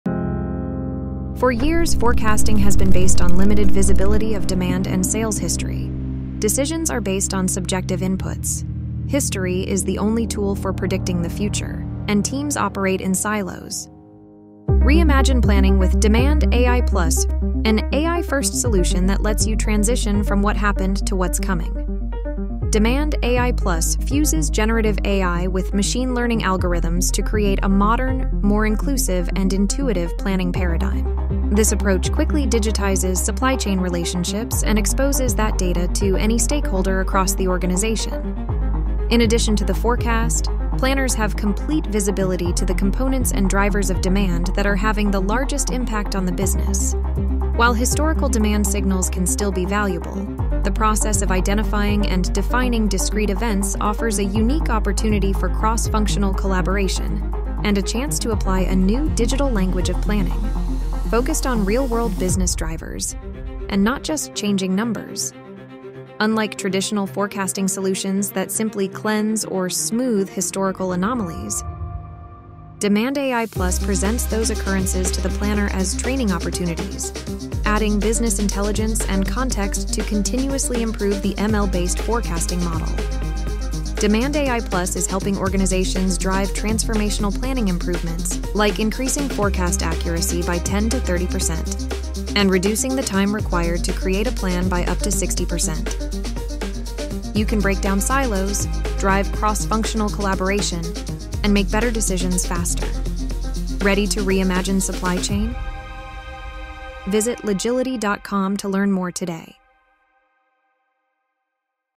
Logility DemandAI+
Reimagine demand planning with an AI-first approach that captures events in real time and creates a language to connect that information to the peaks and valleys of demand. Information is immediately available, routine work is automated, and emotion is replaced by insights. The result? New levels of forecast accuracy, visibility, and speed.
- Reduce forecast error by 10% to 30%
- Decrease work hours to create a plan by up to 60%
- Create a shared digital language